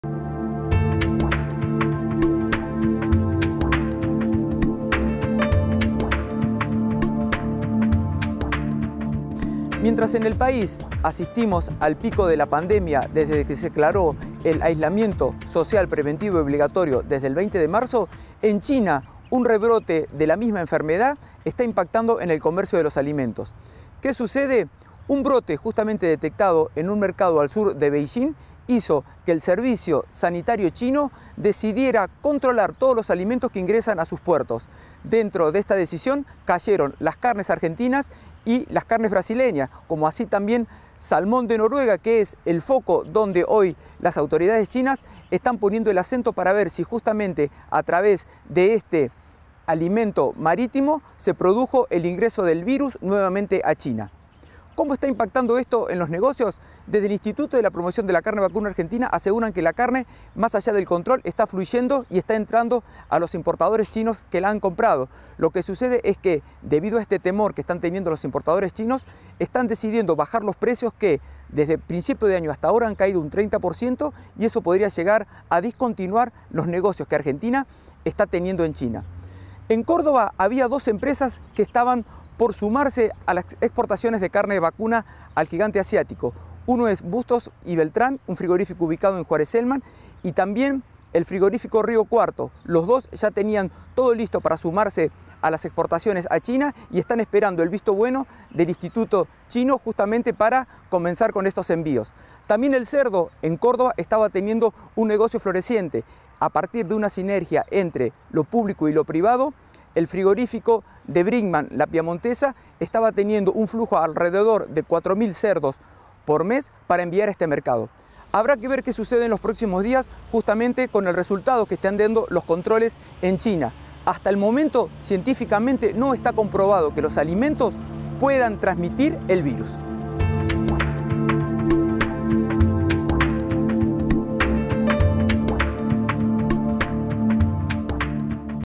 China refuerza el control sanitario a carne argentina
El Gobierno Chino refuerza el control a la importación de carne argentina por un rebrote local de Coronavirus